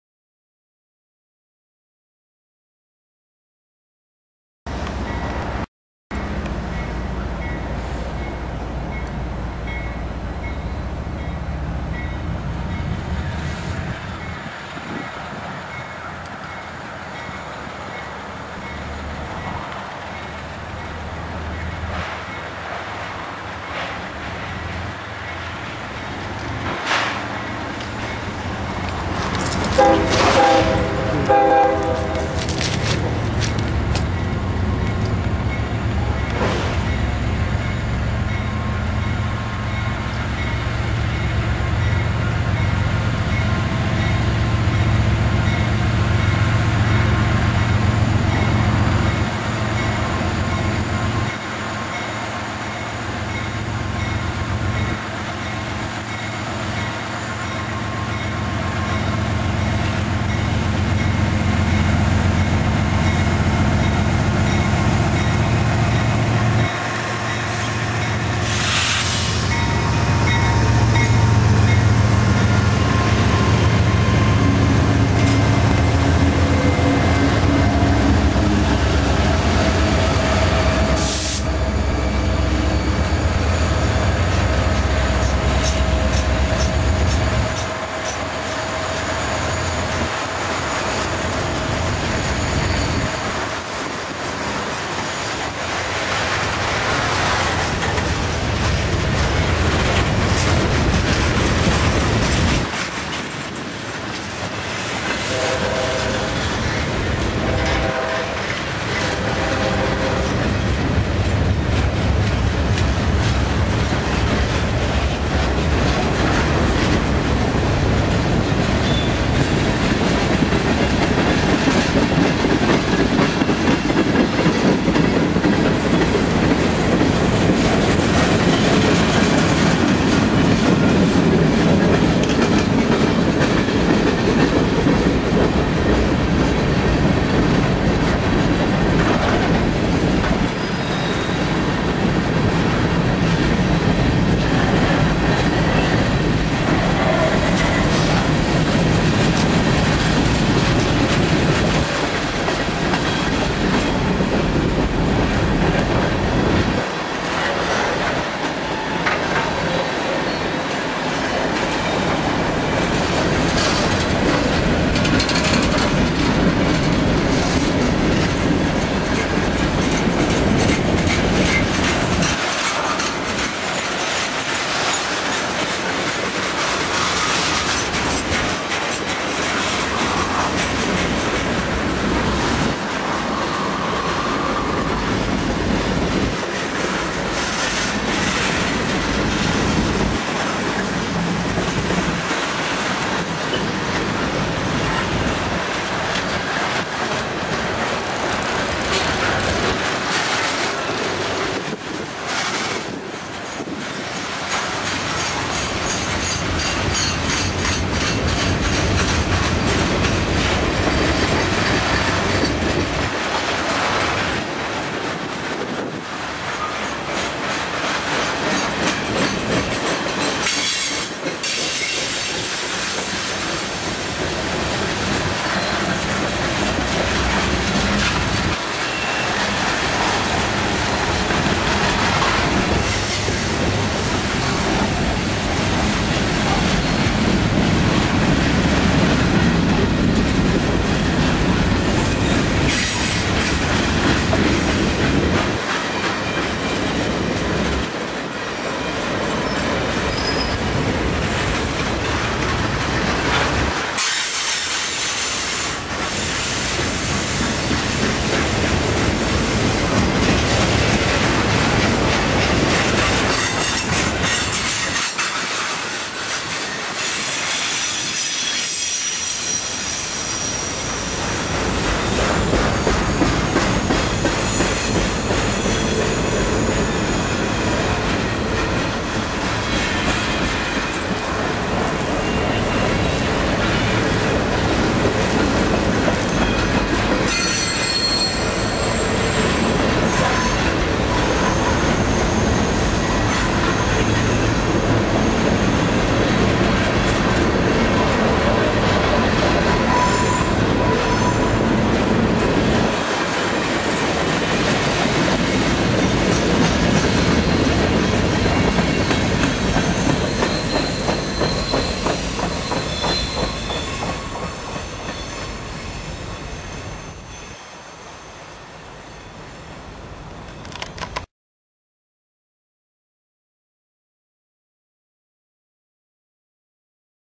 CSX 5434, 90, & 8880 resume trip to Baltimore.
CSX 5434, 90, & 8880 resume trip from Lansdowne to Baltimore, Maryland. New signals.